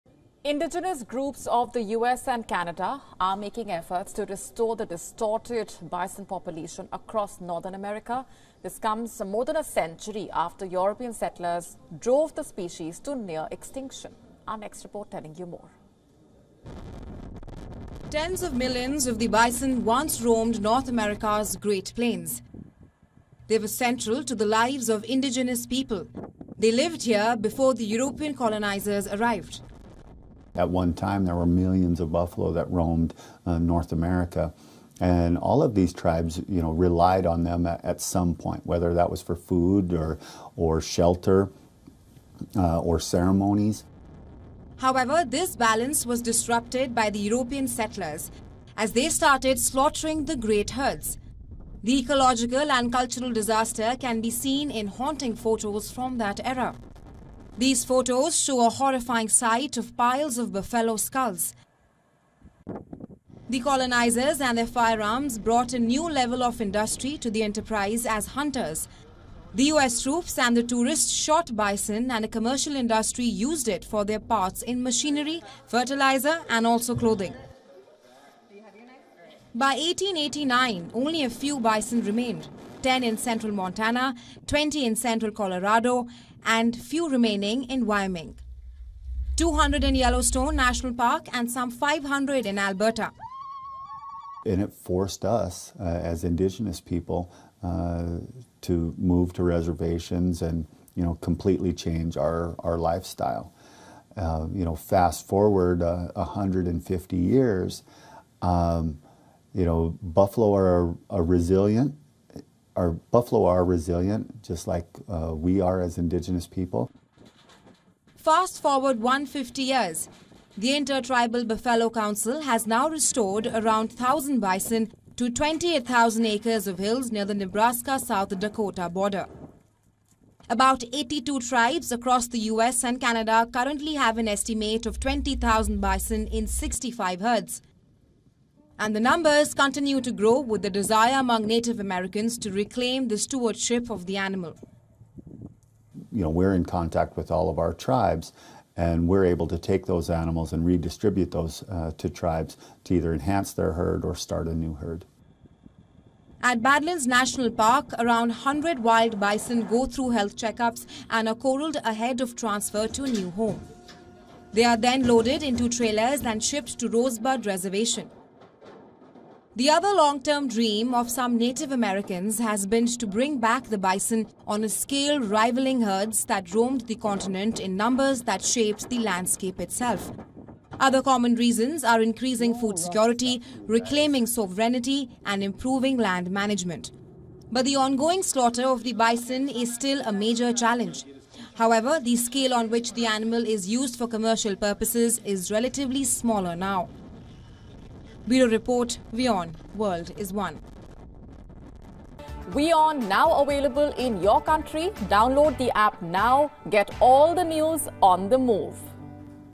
WION Climate Tracker: Efforts to restore Bison across North America | World English News
Native Americans are fighting to restore their legacy. In a bid to restore Bison across North America, native American tribes reclaim stewardship.